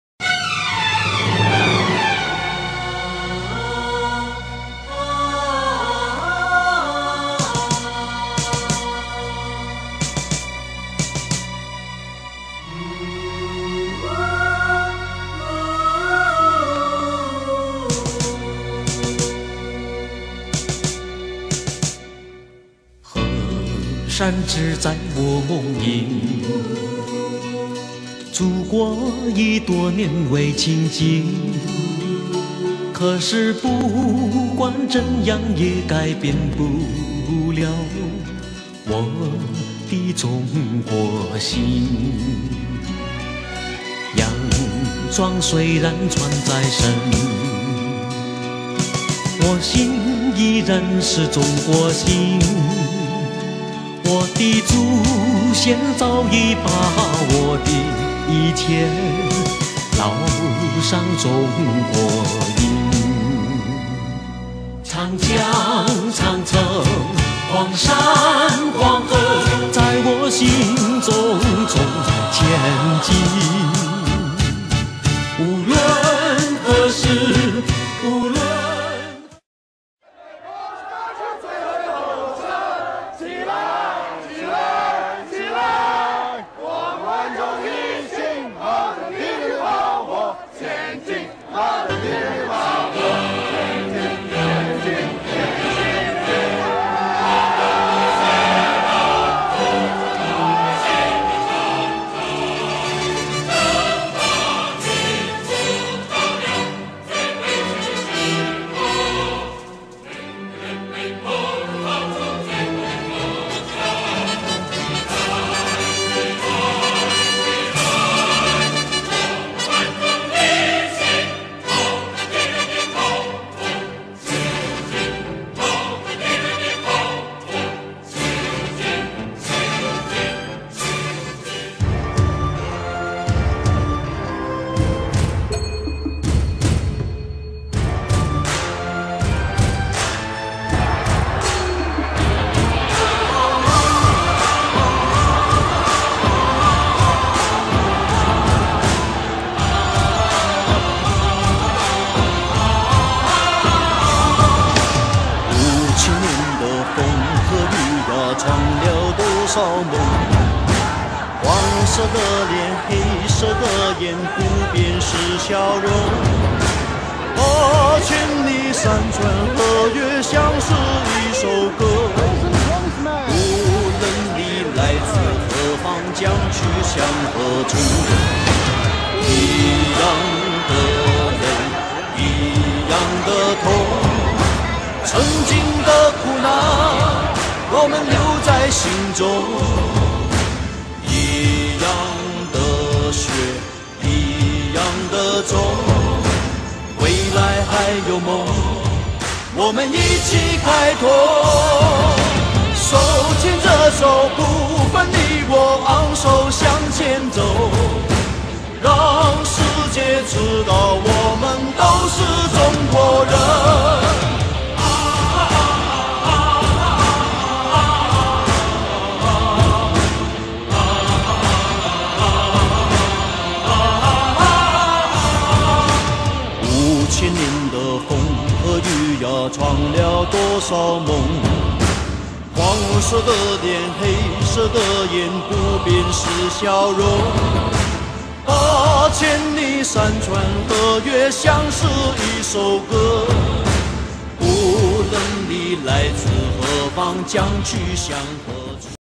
Olympic Torch Relay in London, Britain
Tens of thousands of Chinese students as well as local Chinese community support the upcoming Beijing Olympics 2008.